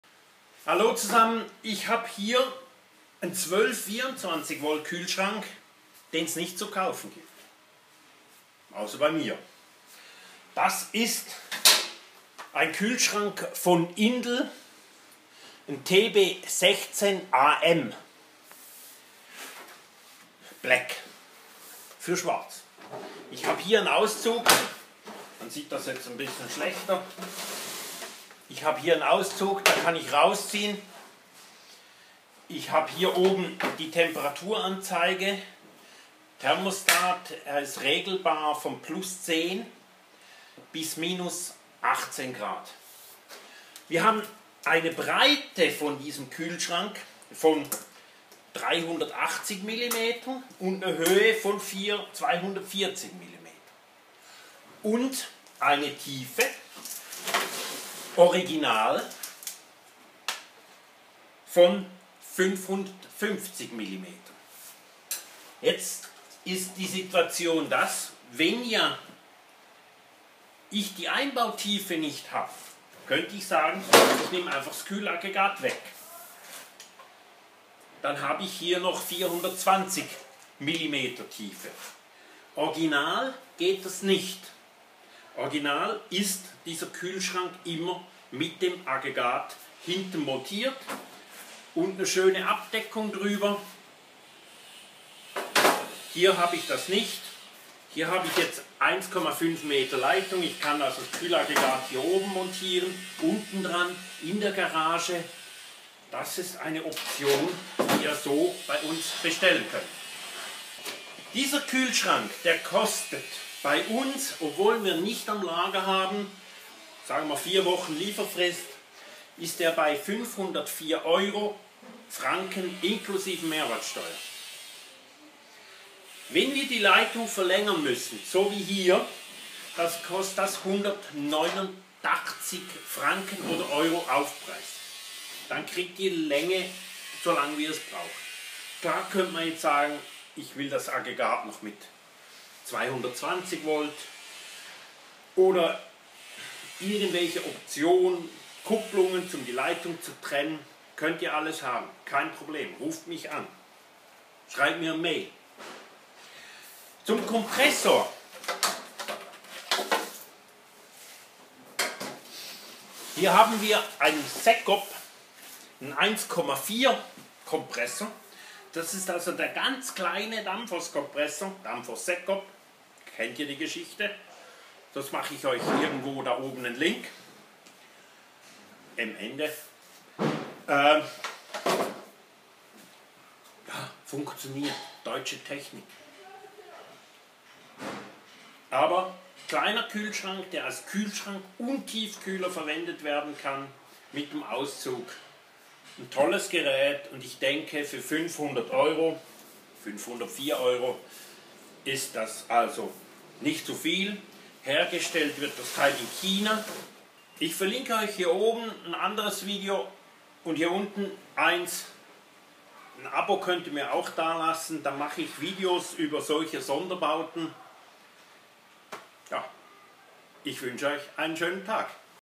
Indel TB 16 AM mit externem Kühlaggregat 12 Volt Auszugsküholschrank für Boot und Wohnmobil
Auszugskühlschrank TB16AM  von IndelB